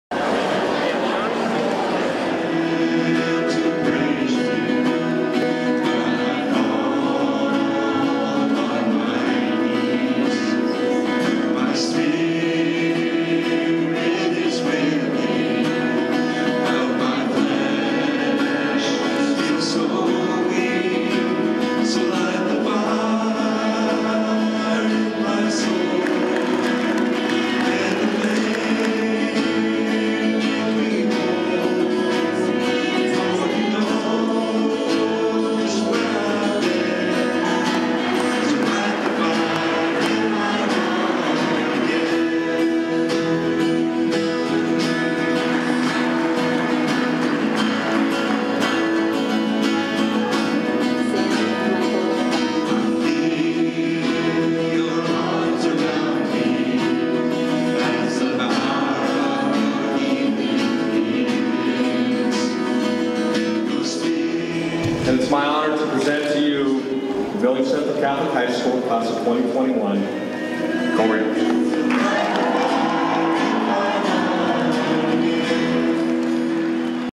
Central sends off Class of 2021
Billings Central High School says goodbye to 83 seniors.